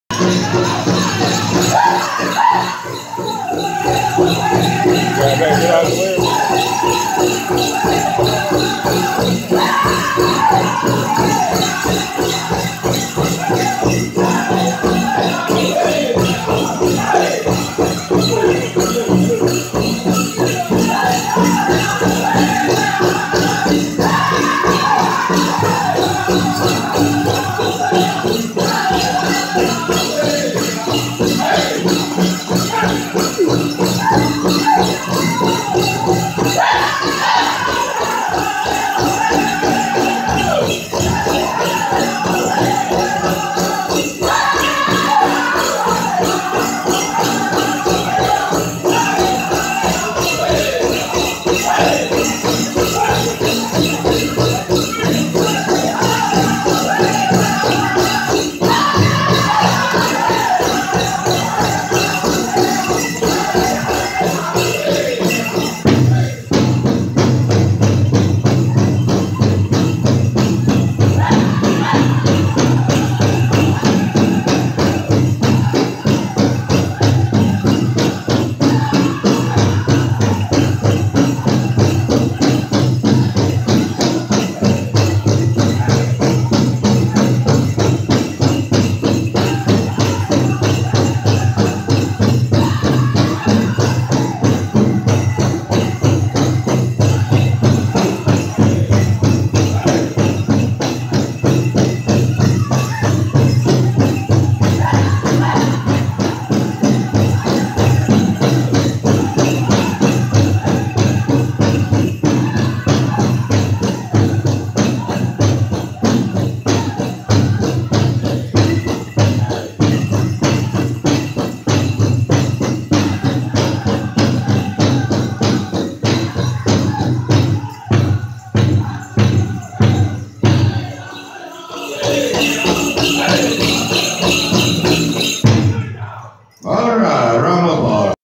Jr. Men's Chicken @ Shooting Star Powwow 2018 (1)
Contest
First song
Saturday Night Live , January 27th, 2018
Shooting Star Casino, Mahnomen